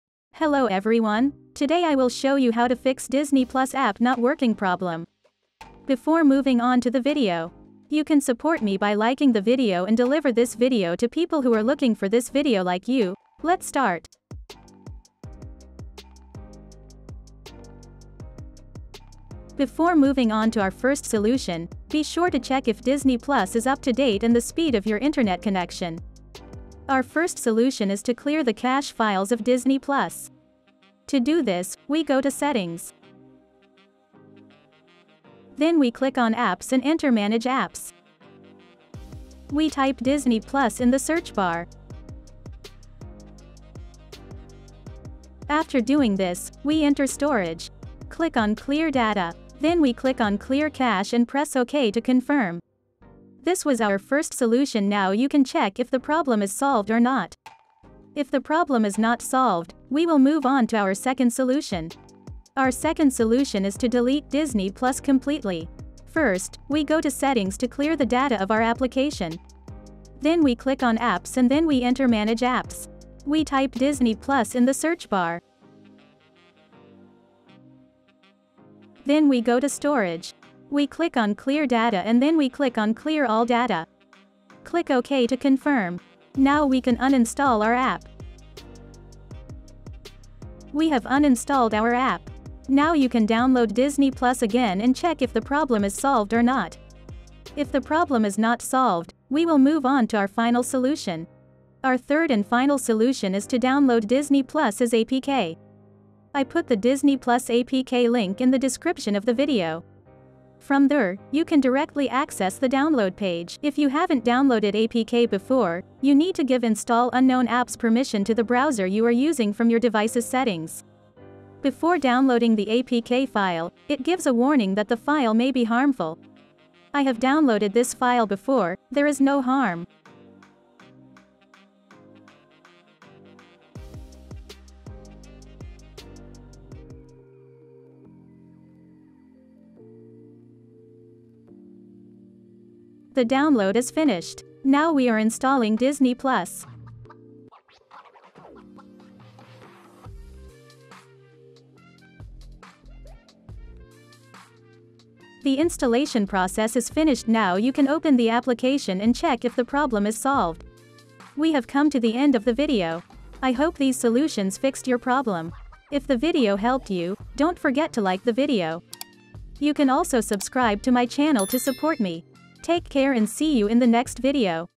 DISNEY PLUS APP NOT WORKING FIX | How to Fix Disney Plus Error Code 42
In this video i’m gonna show you how to fix disney plus app not working. This error is searched on the internet as disney plus error code 42 and disney plus app not loading. And in this video I showed 3 different solutions for disney plus not working fix.

Video Parts:
00:00 How to fix disney plus app not working
00:14 Intro
00:27 1.Solution: Clear the cache files of Disney Plus
01:03 2.Solution: Delete Disney Plus completely
01:42 3.Solution: Download Disney Plus apk
02:42 Ending